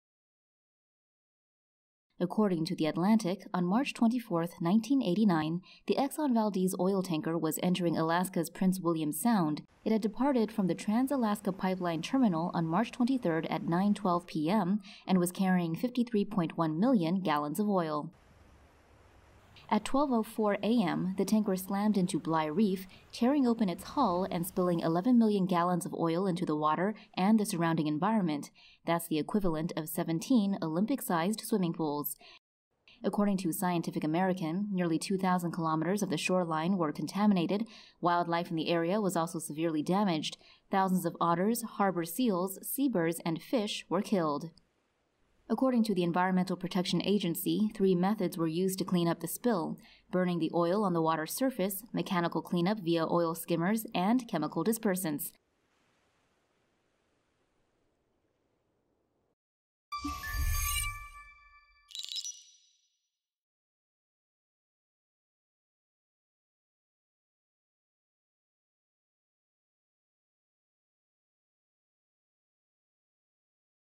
The 1989 US Exxon Oil Spill: Animated explainer
The Exxon Valdez oil spill in Alaska was one of the biggest U.S. oil spills ever to occur, according to the Atlantic.

It's been thirty years since the waters of Bligh Reef in Alaska were contaminated with more than 11 million gallons of crude oil after an Exxon oil tanker impacted the reef's seabed. Today we revisit how the Exxon Valdez Oil Spill happened.

According to The Atlantic, on March 24, 1989, the Exxon Valdez oil tanker was entering Alaska's Prince William Sound. It had departed from the Trans Alaska Pipeline terminal on March 23 at 9:12 pm and was carrying 53.1 million gallons of oil.

At 12:04 am the tanker slammed into Bligh Reef, tearing open its hull and spilling 11 million gallons of oil into the water and the surrounding environment. That's the equivalent of 17 Olympic-sized swimming pools.

According to Scientific American, nearly 2,000 kilometers of the shoreline were contaminated. Wildlife in the area was also severely damaged. Thousands of otters, harbor seals, seabirds and fish were killed.

According to the Environmental Protection Agency, three methods were used to clean up the spill: burning the oil on the water's surface, mechanical cleanup via oil skimmers and chemical dispersants.

High-pressure hot water was also used to clean the surrounding beaches until it was determined that it was cooking microorganisms present in the shores which in turn damaged the soil.

According to a case study by the International Tanker Owners Pollution Federation Limited, Exxon Mobil had to pay $4.3 billion as a consequence of the spill in addition to the $2 billion it spent the first year on clean-up efforts.

The Exxon Valdez oil spill is the second largest spill in history second to the Deepwater Horizon oil spill in the Gulf of Mexico, where an explosion at a BP oil rig killed eleven workers and spilled 53,000 barrels of oil per day for three months.

RUNDOWN SHOWS:
1. Exxon Valdez oil tanker route
2. Exxon Valdez tanker hull breaks open spilling 11 million gallons of oil
3. Shoreline and wildlife damage
4. Methods used to clean oil spill

SOURCES:
The Atlantic, Scientific American, EPA,  International Tanker Owners Pollution Federation Limited